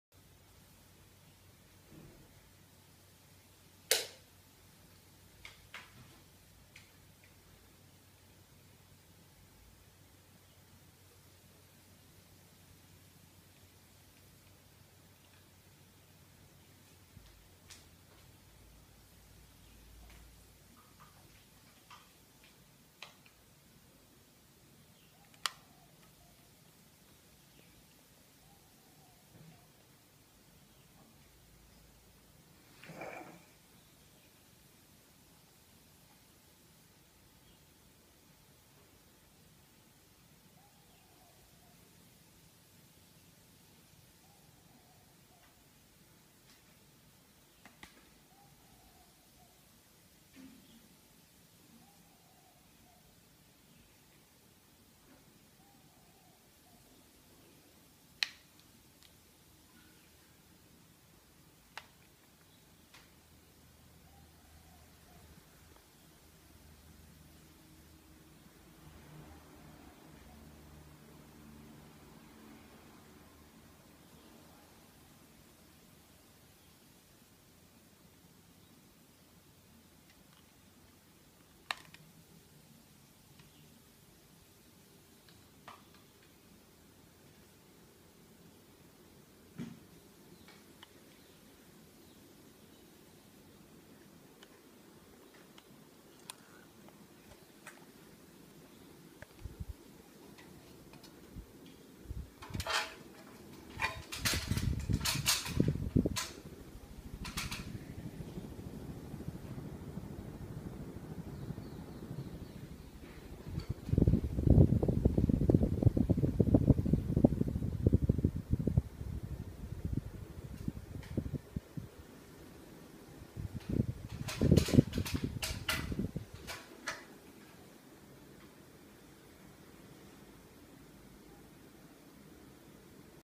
(Shown on all speeds) REPAIRED, Fanimation brand "Benito" ceiling fan (SHORTER VIDEO)
This video is for whoever wants to watch a brief video, rather than a long, detailed video.

We FINALLY had the high speed on this thing fixed. Before, high speed was slower than medium!

Blade diameter: 52" (132cm)
Blade finish: Cherry/Walnut (walnut side showing)
Fan assembly finish: Satin Nickel
Model: FP8003-220
Power rating: 220-240 volts, 50Hz, 2000W
-Power of fan: 80W
-Power of light: 120W (with original bulbs)
Bulb type: E14, G50
Bulb rating: 2 x MAX 60W, 220V
IP20